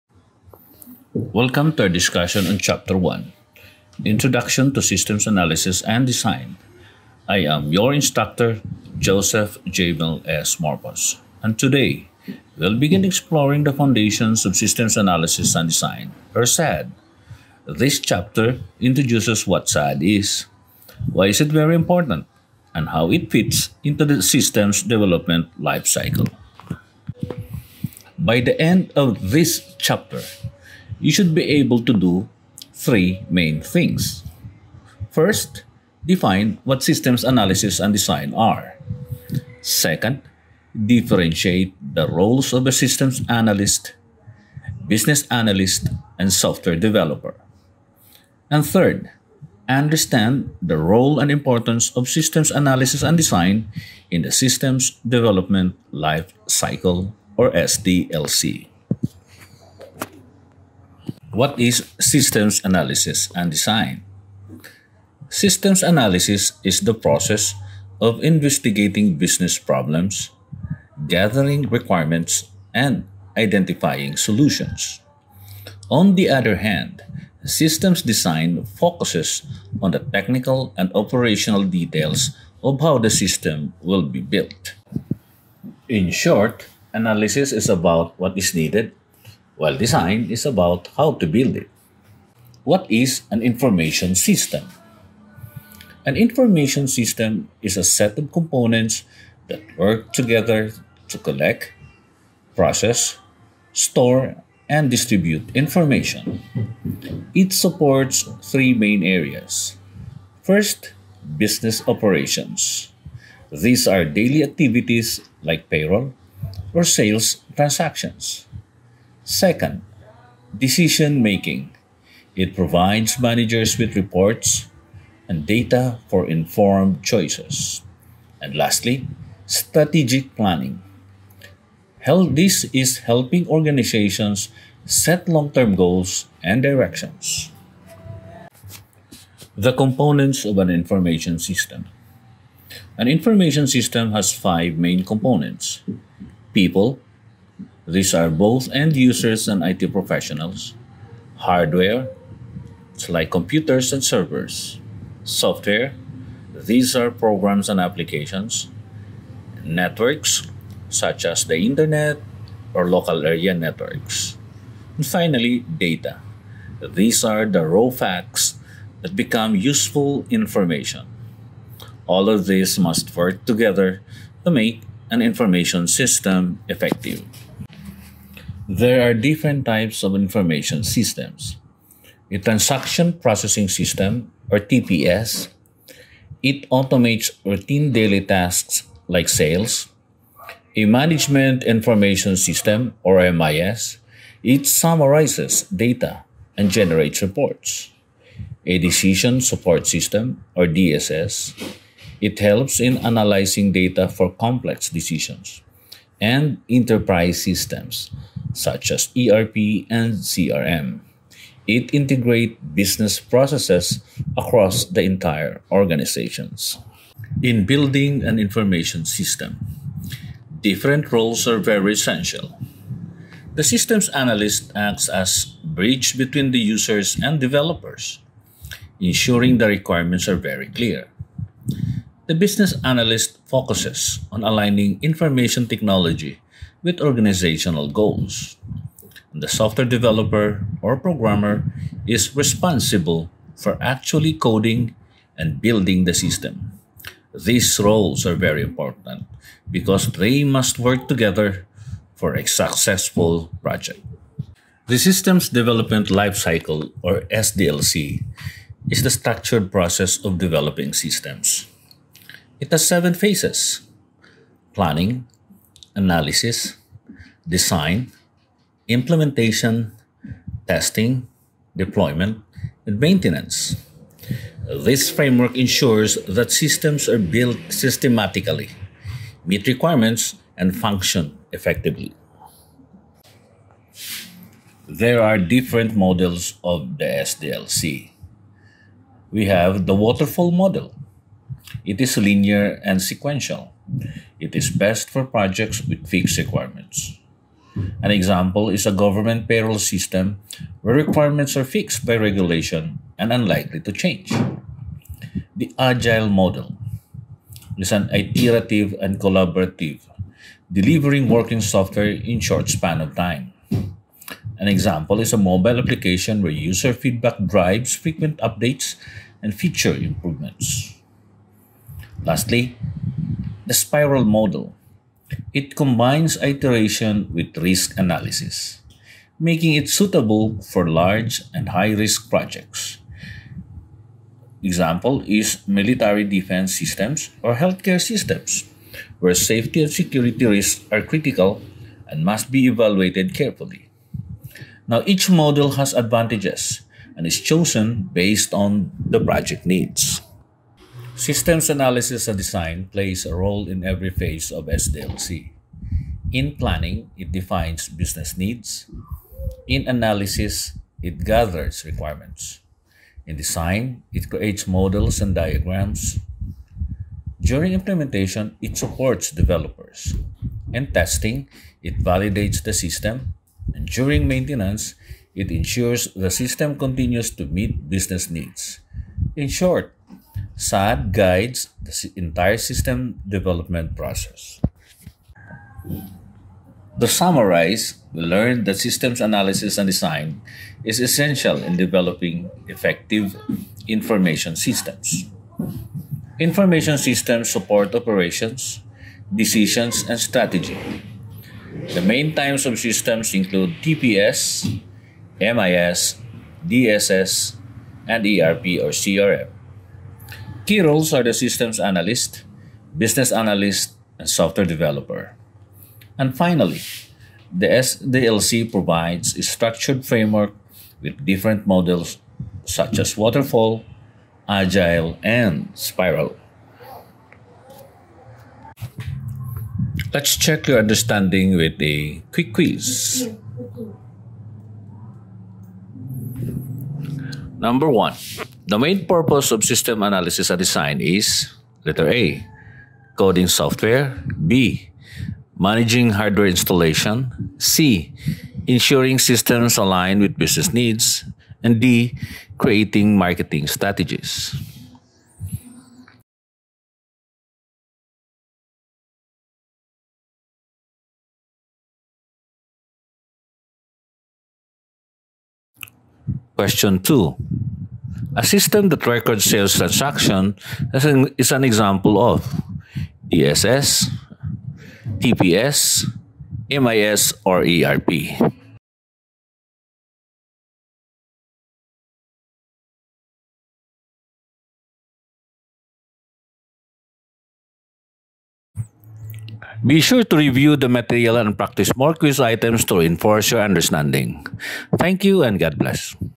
Chapter 1: Introduction to Systems Analysis and Design
Welcome to the first chapter of our Systems Analysis and Design lecture series!
In this video, we explore the foundations of Systems Analysis and Design (SAD) and its importance in developing effective information systems.

What you’ll learn in this video:
Definition of Systems Analysis and Systems Design
Key roles in system development: Systems Analyst, Business Analyst, and Software Developer
Components and types of Information Systems (TPS, MIS, DSS, ERP/CRM)
The Systems Development Life Cycle (SDLC) and its phases
Popular SDLC models: Waterfall, Agile, and Spiral
The role of SAD in guiding each phase of system development

This chapter provides the groundwork for understanding how organizations design and build systems that support business operations, decision-making, and strategic planning.